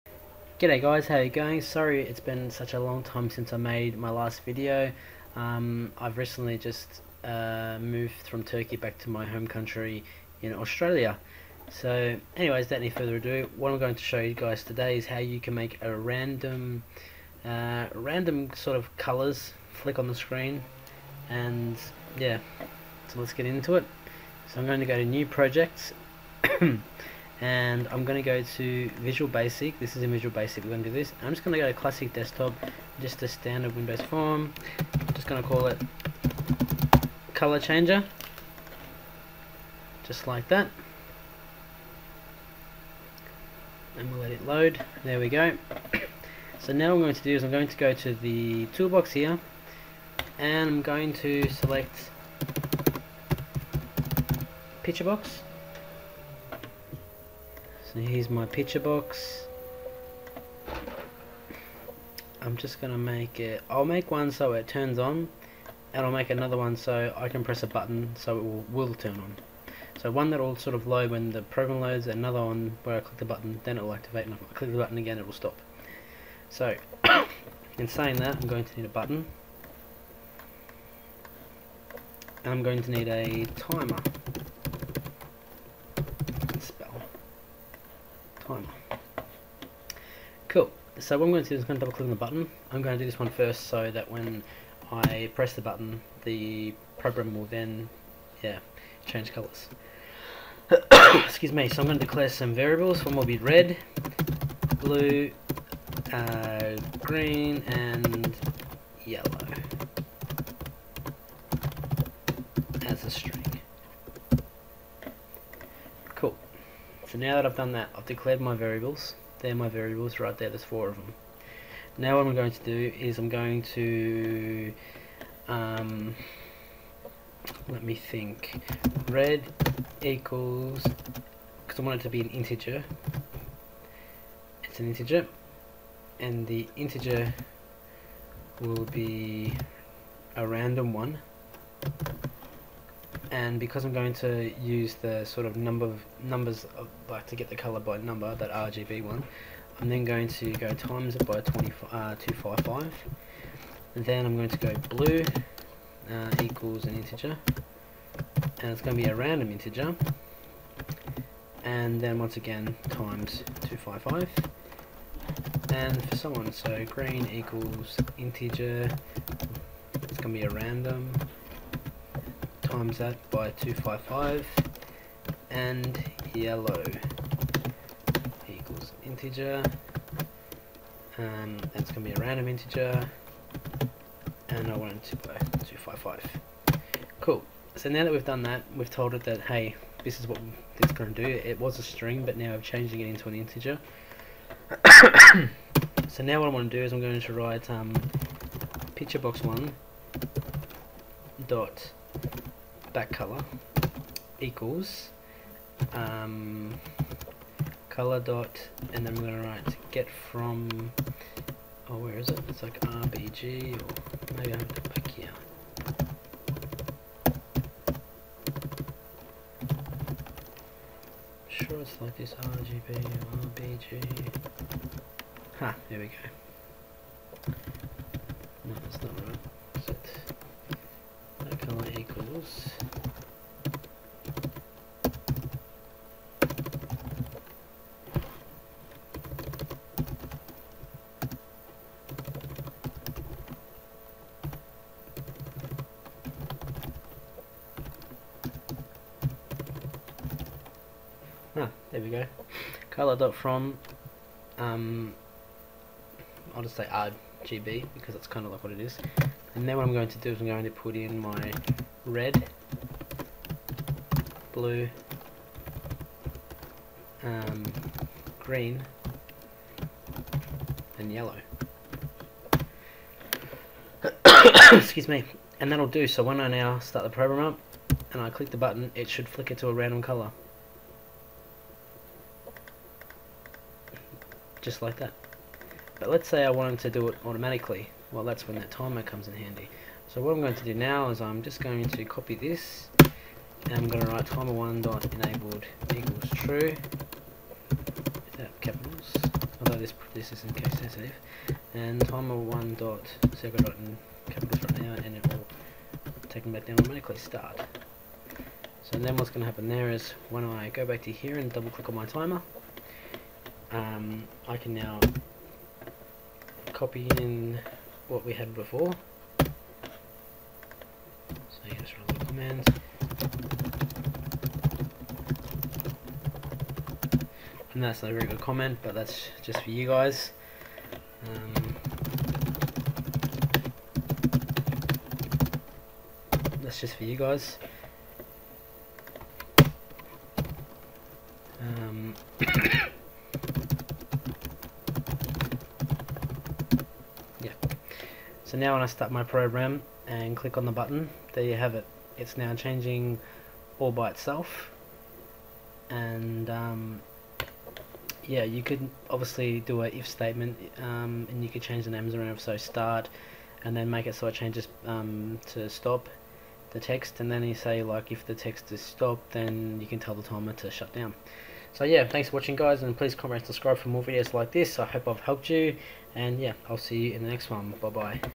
HOW to get a random colour changer in VB.NET
G'day guys, here is a quick video on how to get a random colour changer in VB.NET.

If you have any questions, let a comment.